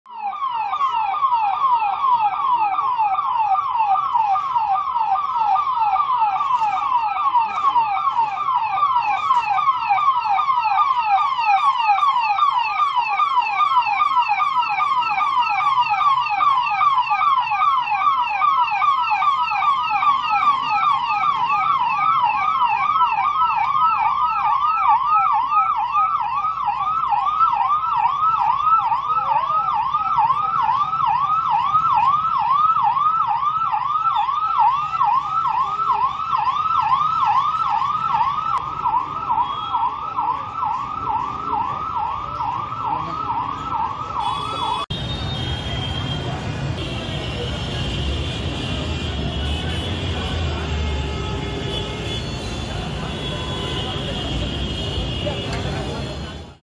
CAA implemented: Tightened security, flag march in Delhi
Citizenship Amendment Act implemented: Tightened security, flag march in Delhi